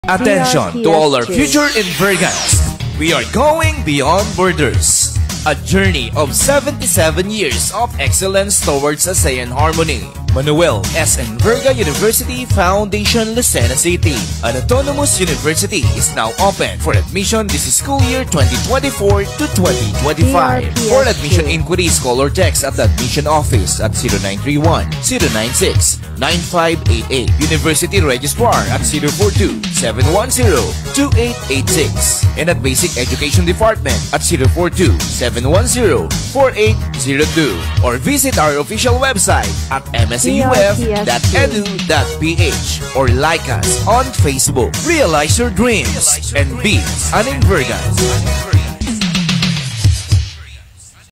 Manuel S. Enverga University SY 2024-2025 Radio Ad 2024 51s (Gumaca, Quezon)
This is the 2024 Provincial Radio Ad for Manuel S. Enverga University SY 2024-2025 in Gumaca, Quezon.

DRPS Two 6857: Manuel S. Enverga University SY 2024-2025 Radio Ad 2024 51s (Gumaca, Quezon)